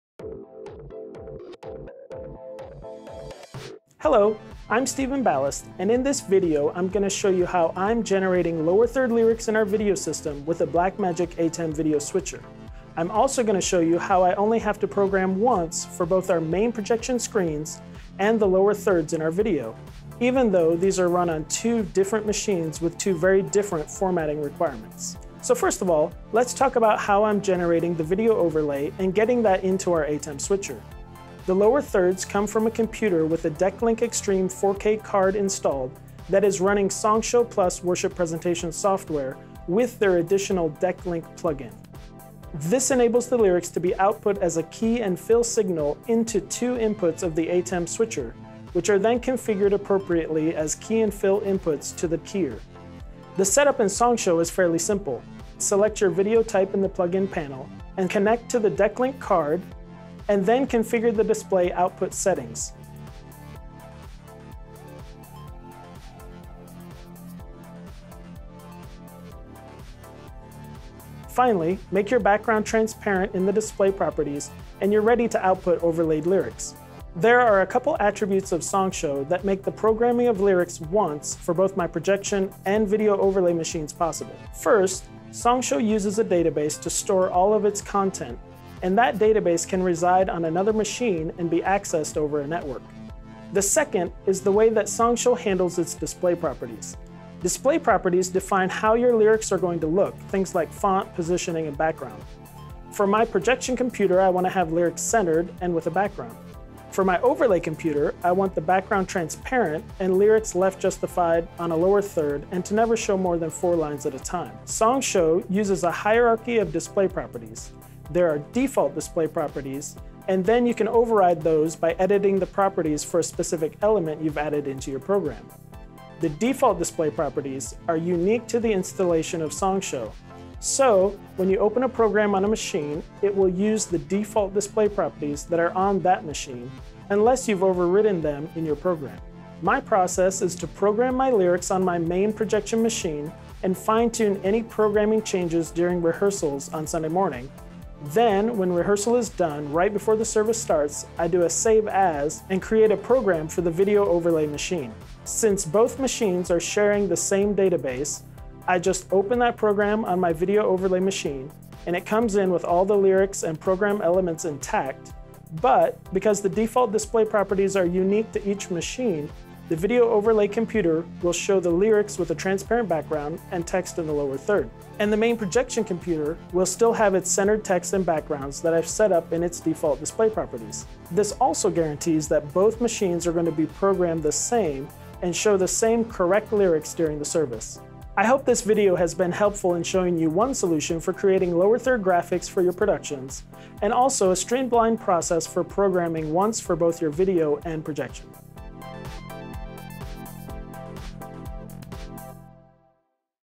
Creating Lower Third Lyrics with an ATEM Switcher
Equipment in this video:
Blackmagic ATEM Television Studio Switcher
Blackmagic ATEM Production Studio Switcher
Decklink 4k Extreme Card

In this video I describe how I create lower third lyrics using an Blackmagic Designs ATEM video switcher and Song Show Plus worship presentation software.  A Decklink Extreme 4k card is used to generate the Key and Fill signals needed from the PC running SSP.